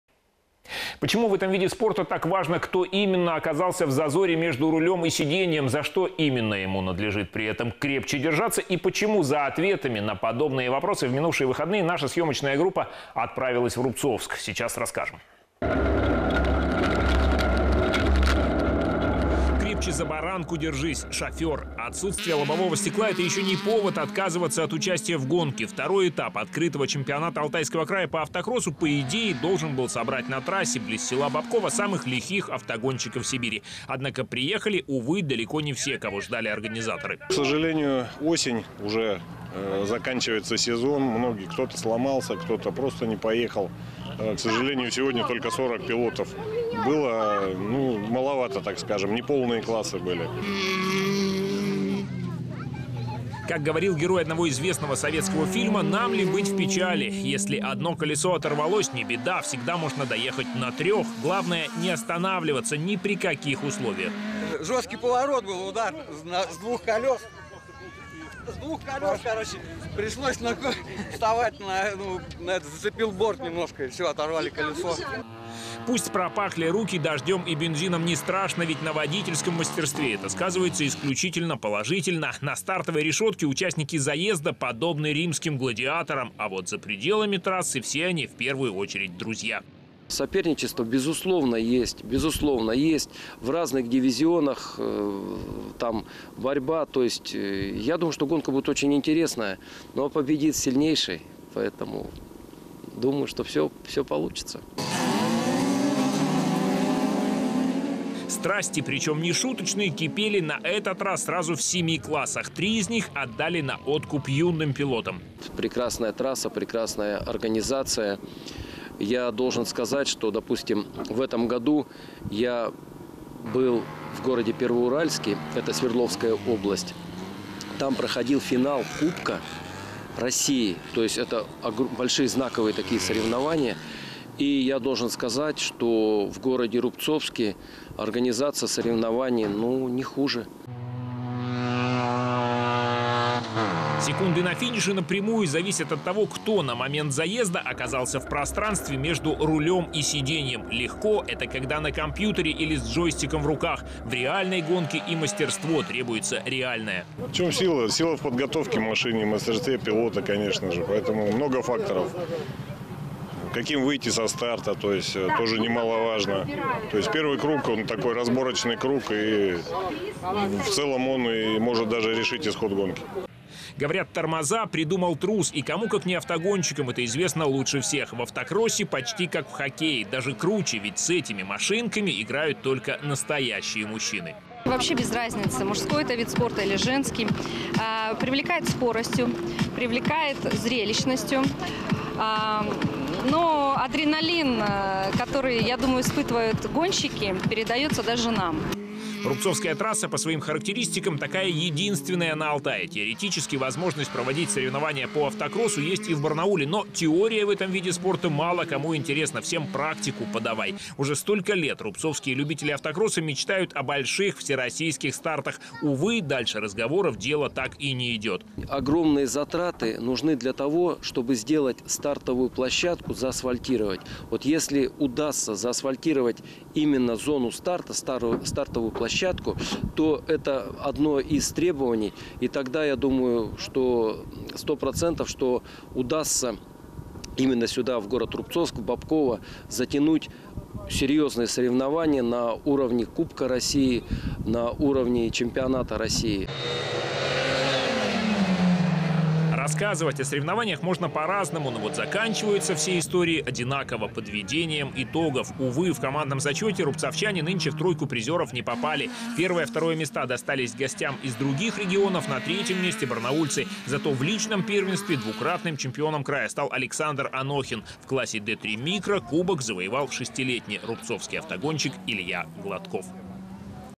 Автокросс в Рубцовске
Кто придумал тормоза и за что надлежит как можно крепче держаться настоящему шоферу: ответы на эти вопросы прекрасно знают участники чемпионата края по автокроссу, итого которого в октябре подвели в Рубцовске.